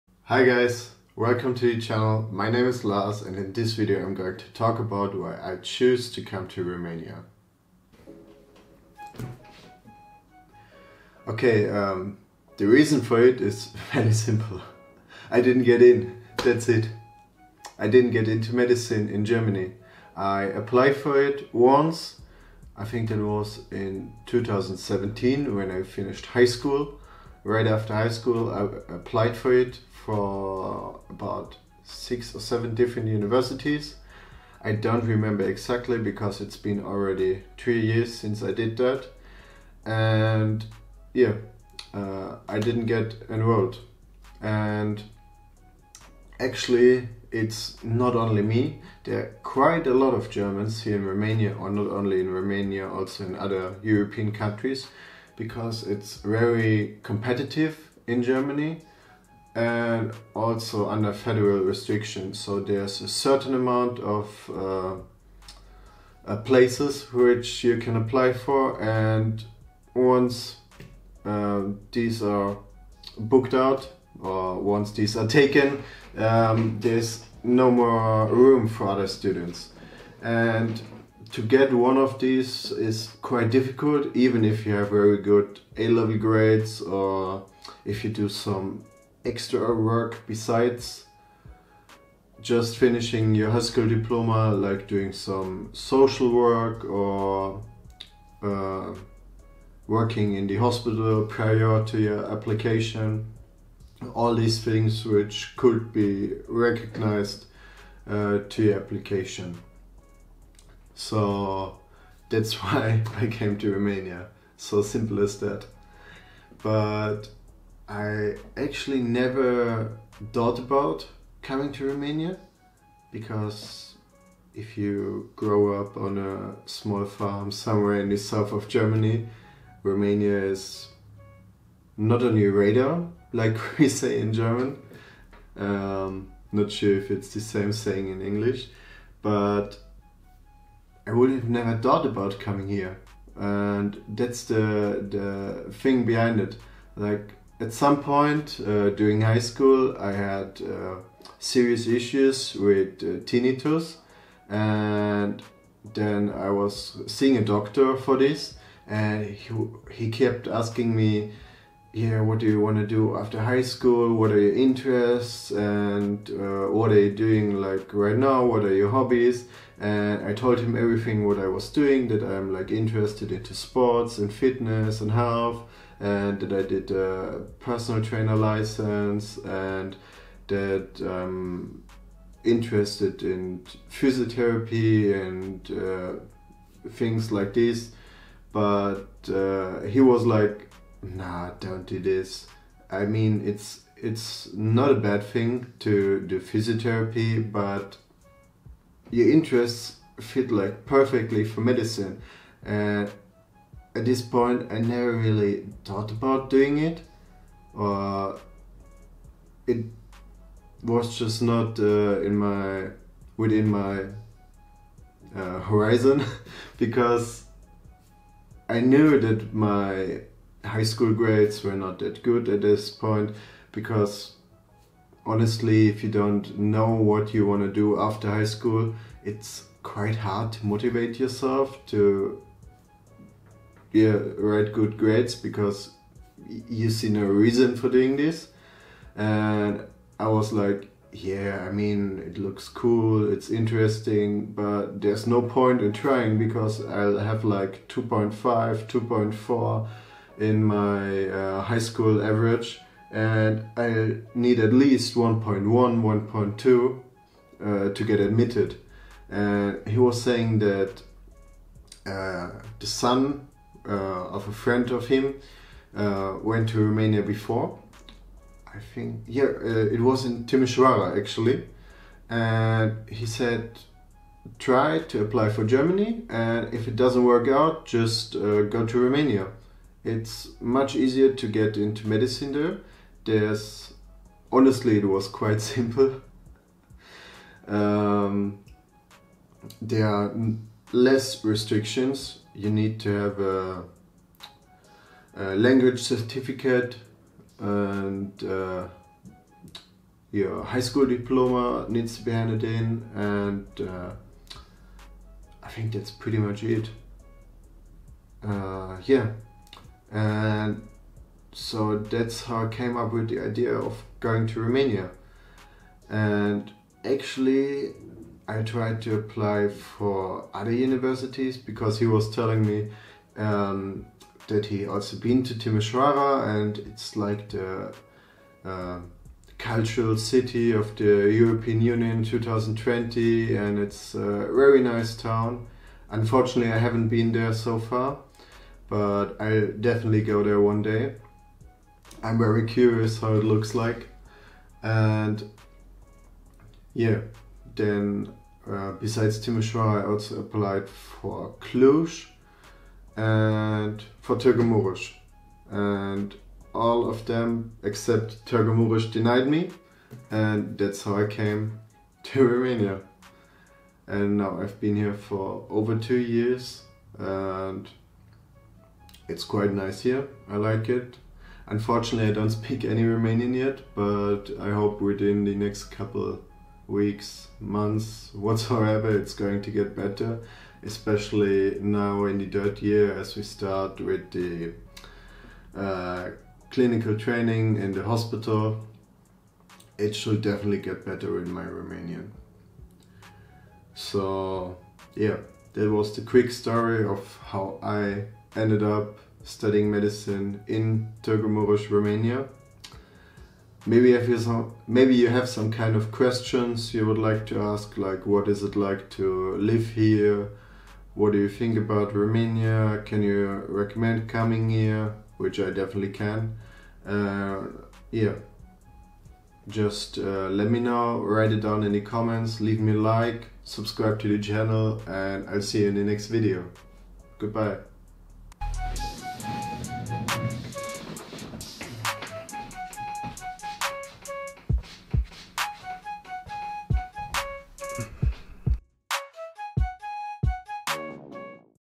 Why I choose to come to ROMANIA
In this video, I am talking about the reasons that brought me to Romania.